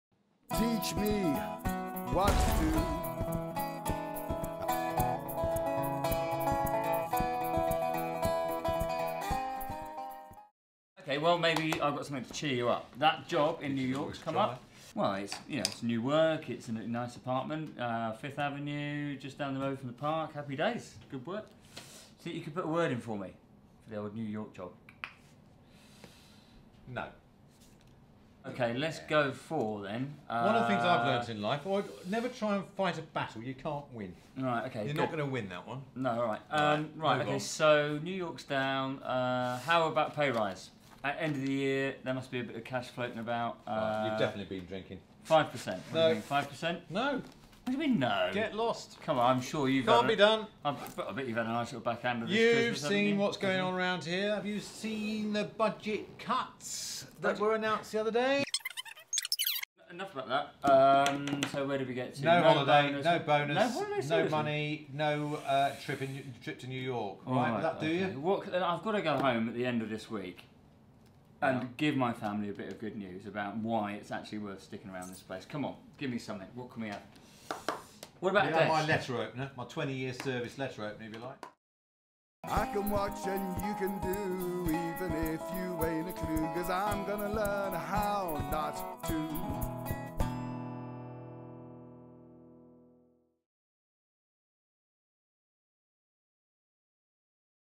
Negotiating: How NOT to
This is how NOT to do it.  Do NOT try this at home!

The idea is that as you watch it you ask yourself what the mistakes are.  There are at least 20 in the full length version.

This trailer gives you a feel for the approach.  For the full length version and the "gaffe guide" which goes with it,